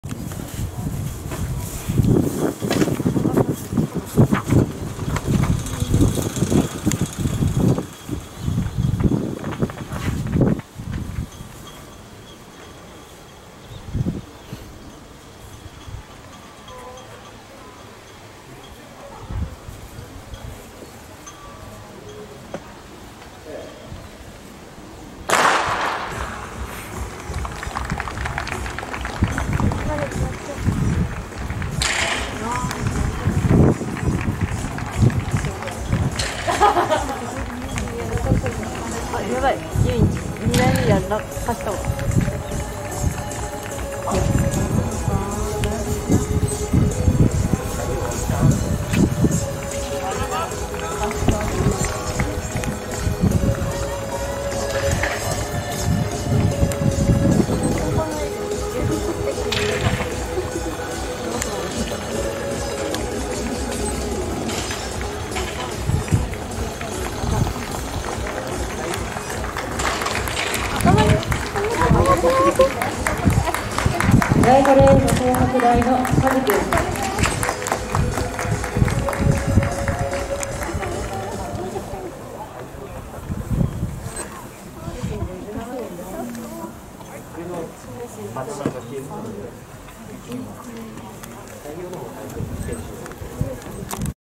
【2022東北インカレ】男子400ｍH予選3組
75回東北インカレ　2022年6月4日　岩手県・北上総合運動公園北上陸上競技場

5着　津田典秀(3)　1:07.43